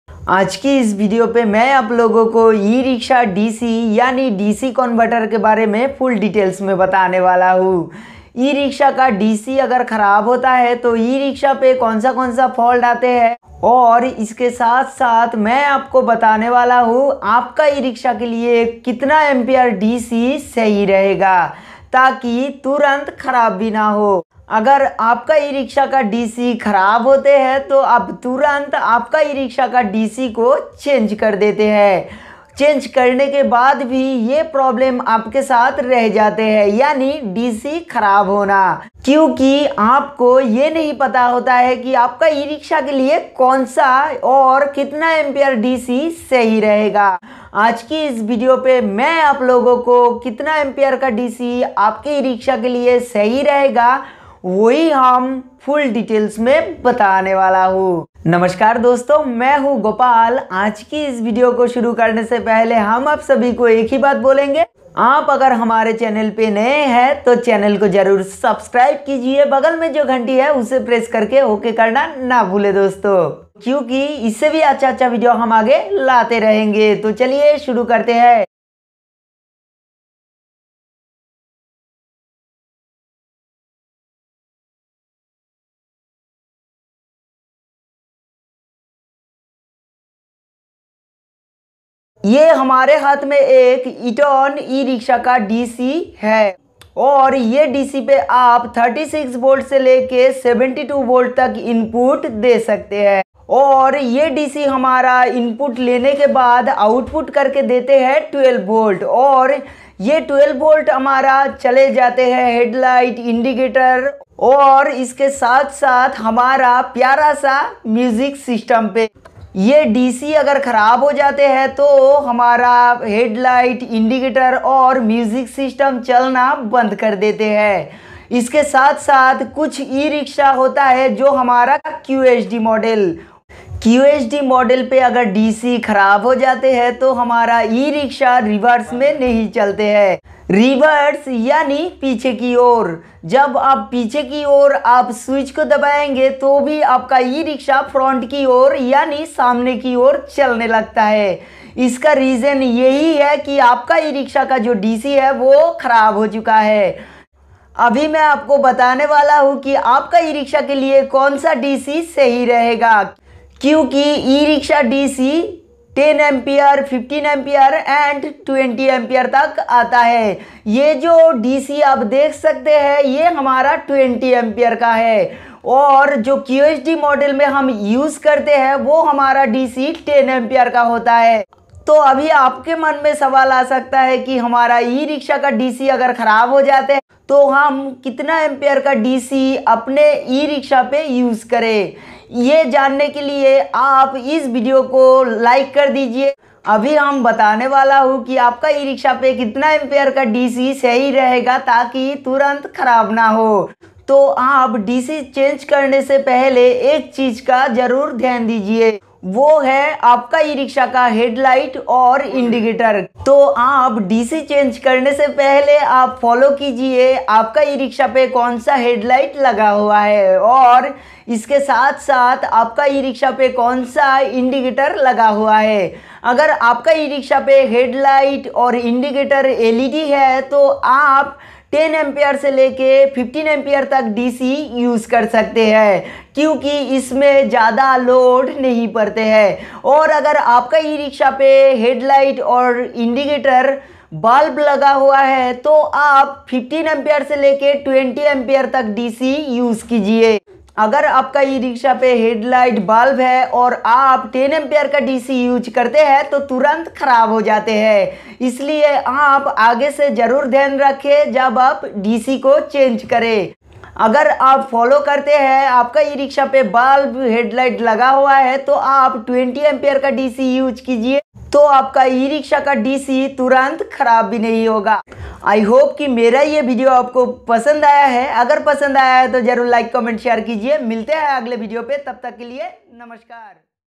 erickshaw DC full details e rickshaw per kaun sa Kitna ampere DC Sahi Hai full details in hindi
Hii
I am gopal barai
 aur aap hi aapka Apna YouTube channel Gopal teach 4 U pe
Aaj ki is video per main bataya hun e rickshaw DC ke bare mein Kitna volt input hota he  or output volt kahan per jata hai aur aapkie rickshaw ke liye kaun sa dc aur Kitna empier ka DC Sahi Rahega Taki jyada tikau ho sake